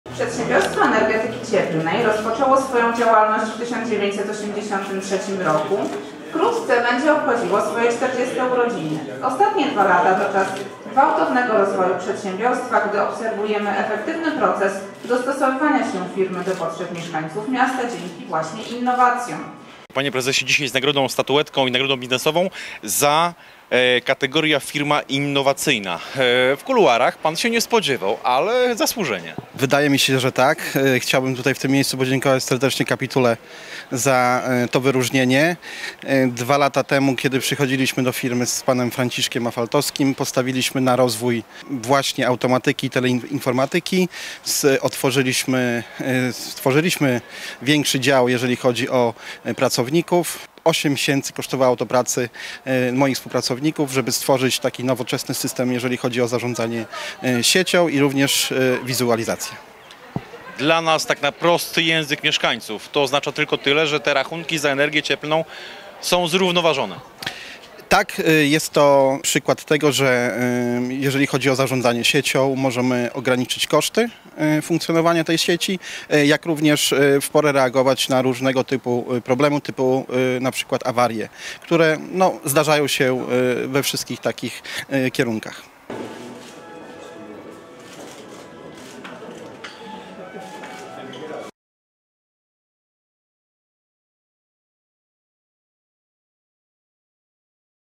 Innowacyjna Firma 2020 - PEC Sp. z o.o. Biała Podlaska
Bialskie Przedsiębiorstwo Energetyki Cieplnej w Białej Podlaskie otrzymało statuetkę Innowacyjna Firma 2020.